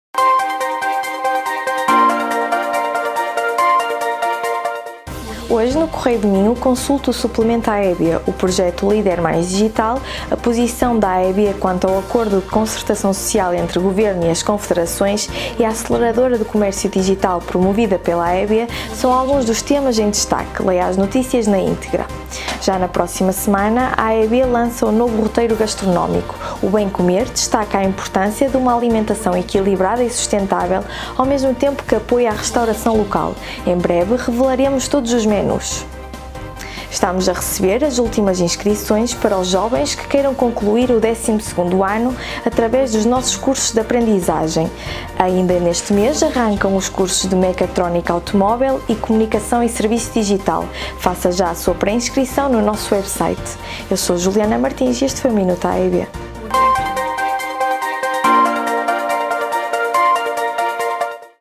Minuto AEB 07/10
MinutoAEB O vídeo do '𝐌𝐢𝐧𝐮𝐭𝐨 𝐀𝐄𝐁' com todas as novidades desta semana já está disponível.
▶️ Em destaque:
📌Suplemento AEB no Correio do Minho
📌Novo roteiro gastronómico 'Bem Comer' | 14 a 31 de outubro
📌 Últimas vagas - Cursos Aprendizagem